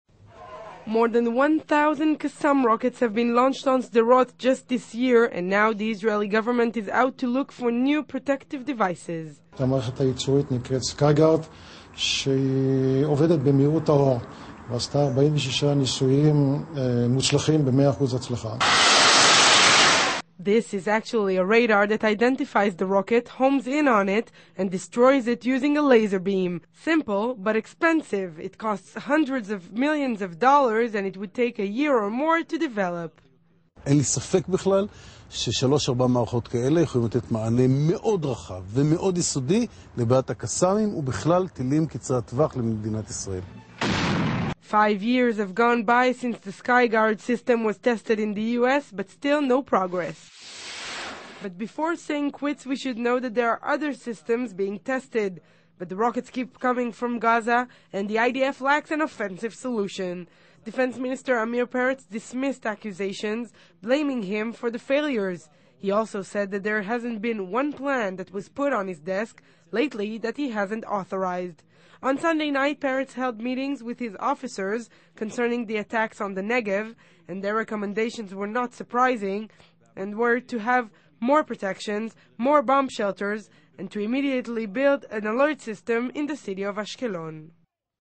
Laser Against Kassam Rockets
As Kassam rockets continue to fall daily on Sderot, the defense forces are trying to come up with creative solutions to the rocket threats. One of them is a laser beam that was developed to track rockets upon launch and destroy them.